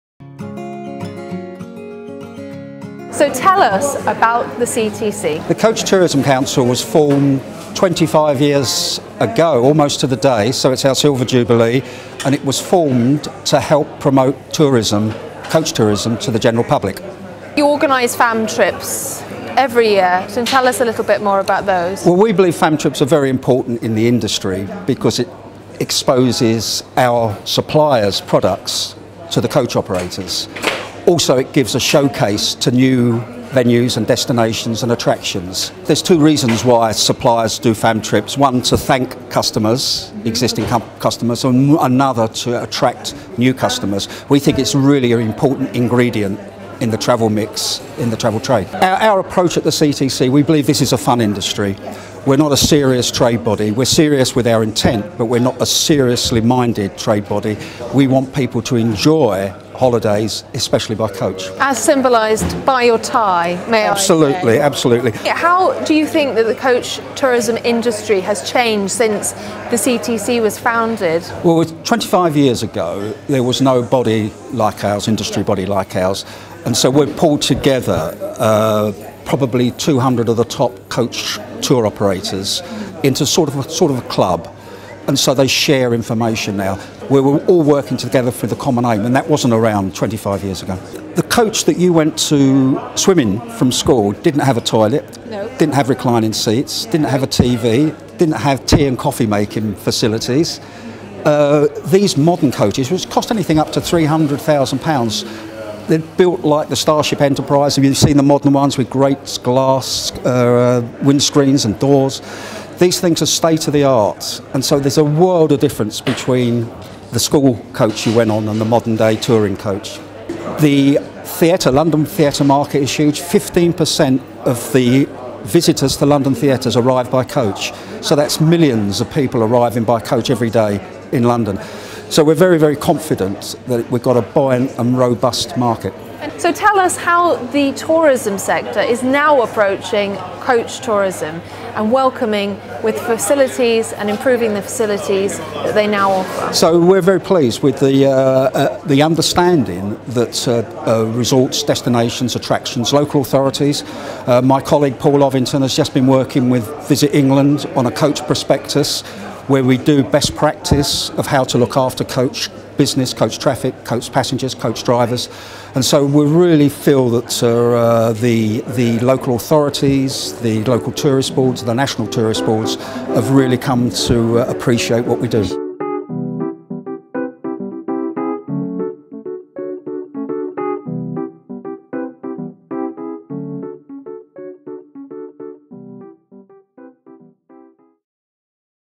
The Pass: Interview with Chris Wales of Coach Tourism Council. Metropolis Multimedia.
The Pass: Interview with Chris Wales of Coach Tourism Council.

Editor, Rebecca McWattie, gains more insight into the coach industry from Christopher Wales, Chief Executive of Coach Tourism Council

Camera and production: David Berman. Copyright: Metropolis Multimedia
unauthorised usage not allowed.